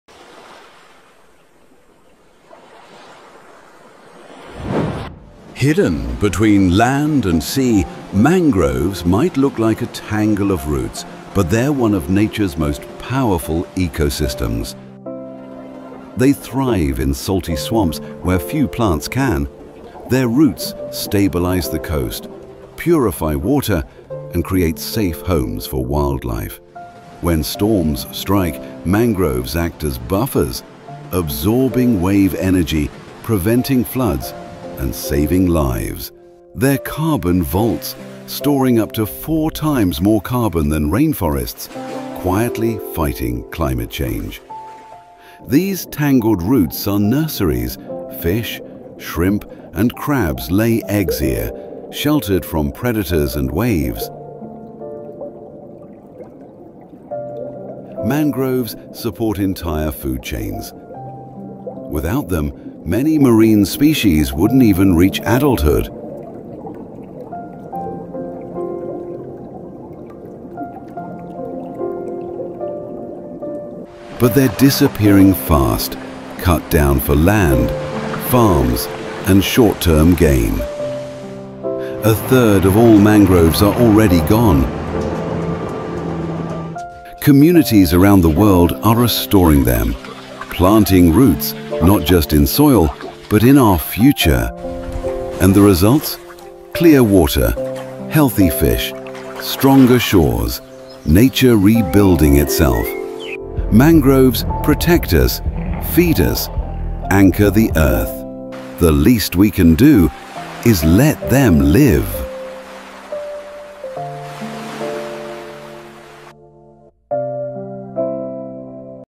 This Nature Documentary Was Created by AI — And It’s Mind-Blowing! 😨🔥
What if nature could fight storms, store carbon, and protect entire coastlines — all on its own?

This is the world’s first AI-generated nature documentary exploring the untold power of mangroves — the forests that grow from the sea and hold back disaster.

Created entirely using AI visuals, voice, and editing, this documentary reveals why mangroves may be Earth’s most underrated superheroes.

🌍 No cameras. No crew. Just intelligence.

🧠 Learn. Share. Protect.